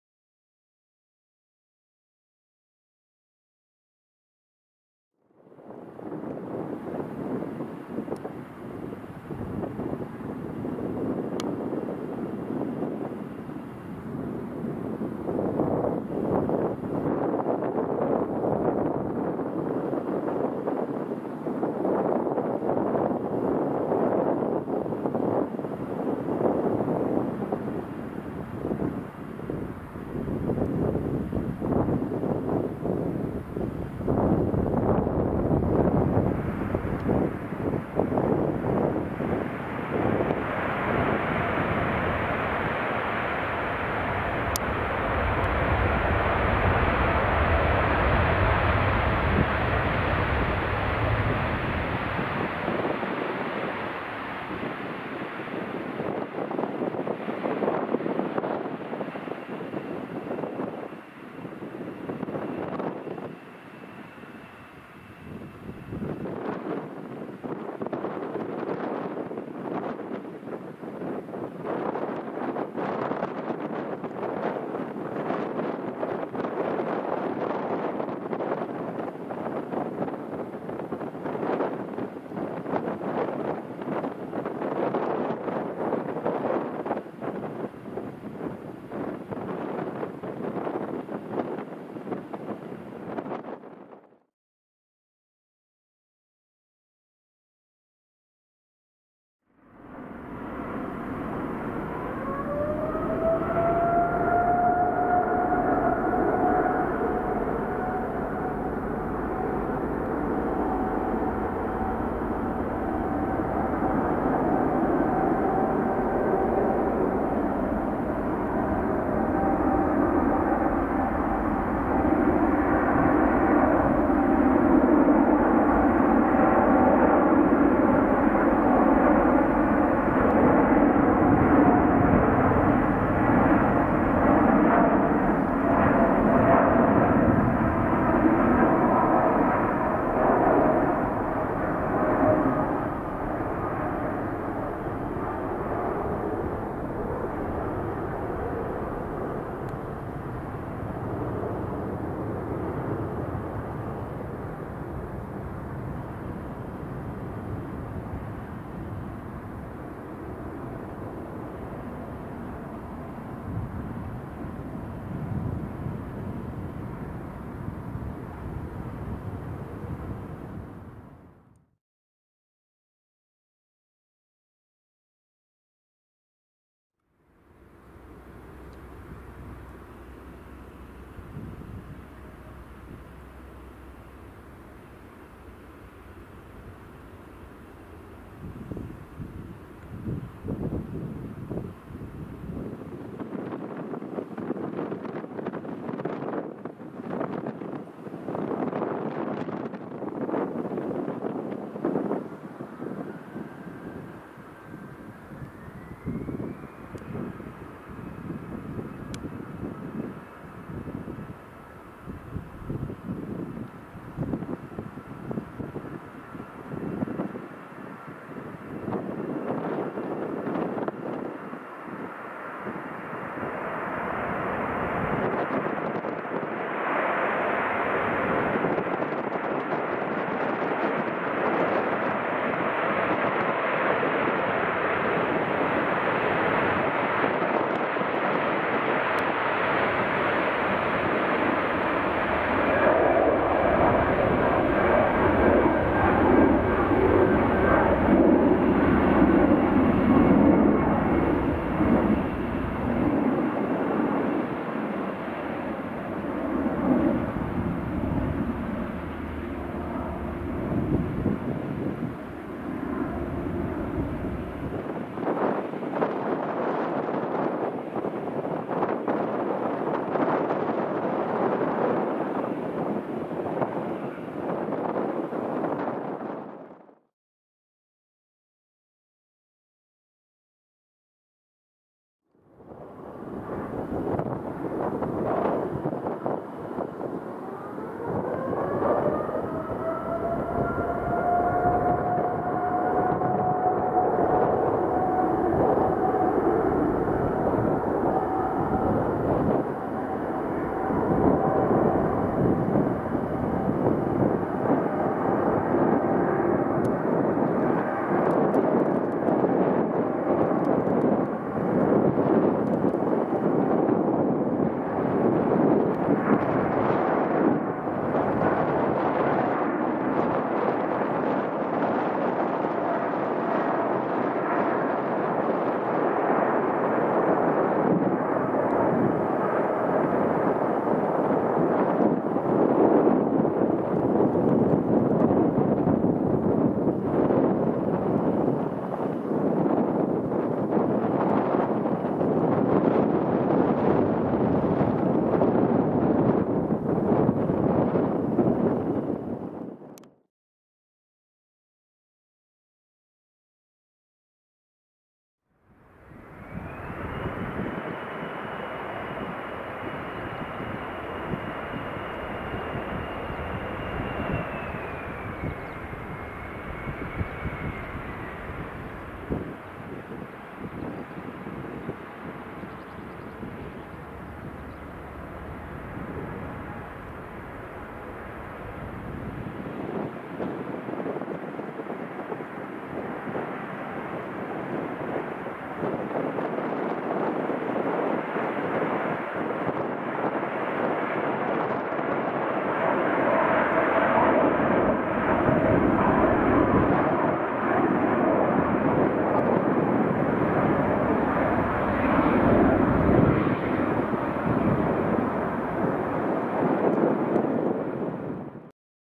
[4K] PARIS CDG Plane Spotting Compilation 2019 - AIR FRANCE B787-9
1- AIR FRANCE B787-9 (F-HRBA) / FROM BOGOTA
2- AIR FRANCE B787-9 (F-HRBE) / TO BOGOTA
3- AIR FRANCE B787-9 (F-HRBC) / FROM OSAKA
4- AIR FRANCE B787-9 (F-HRBG) / TO OSAKA
5- AIR FRANCE B787-9 (F-HRBD) / TO DETROIT